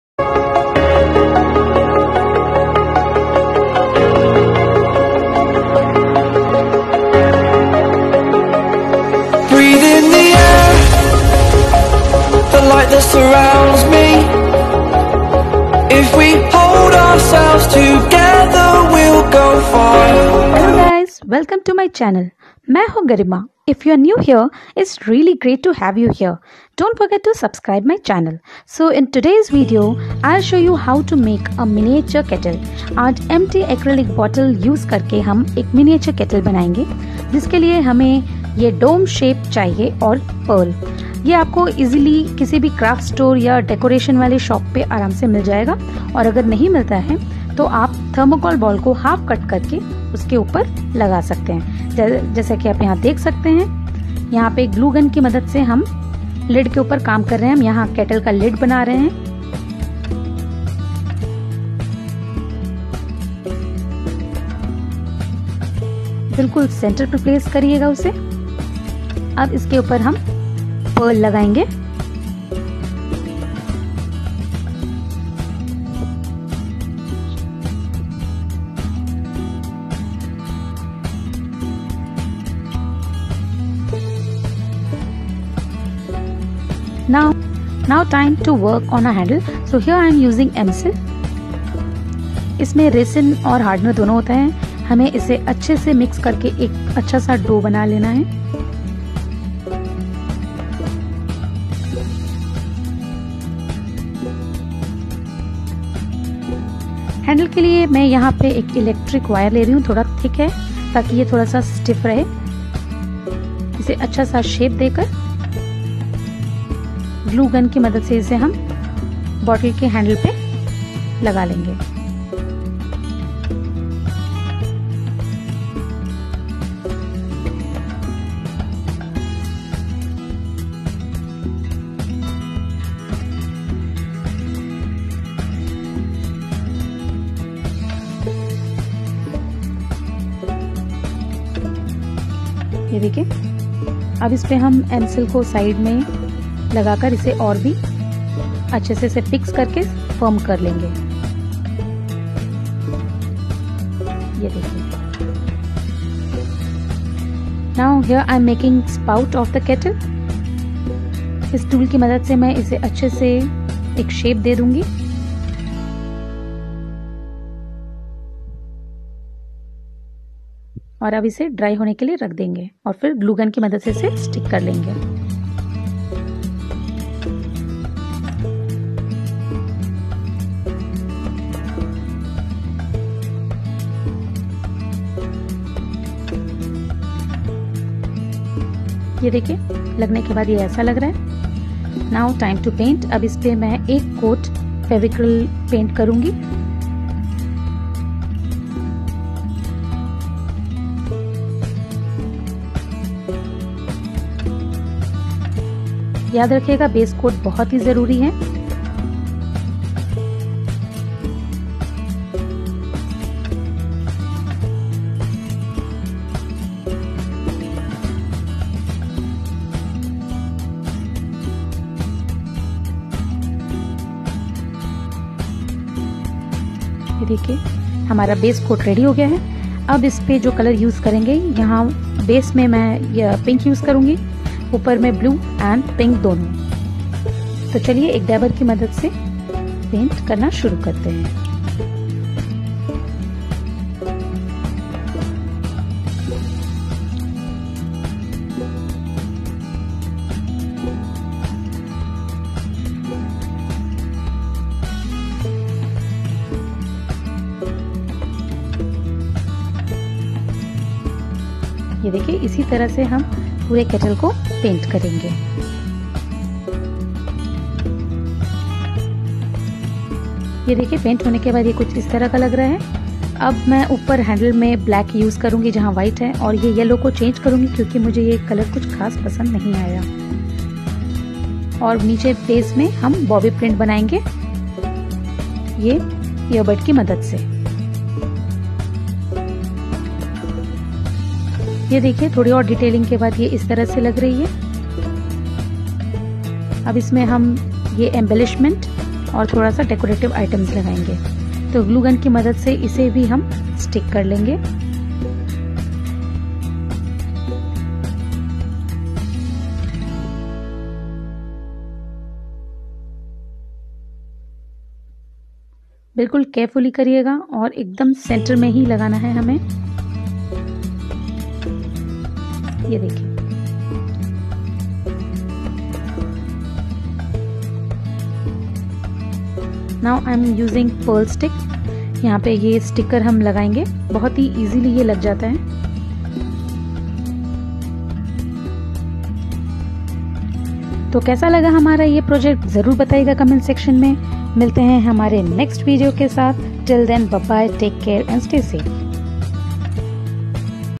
DIY- Miniature Kettle | Reuse of empty acrylic bottle
Hey guys! I am Garima. It's really great to have u here. In this video I have shown how we can reuse empty acrylic bottle and make a beautiful kettle for home decor. I hope you liked it.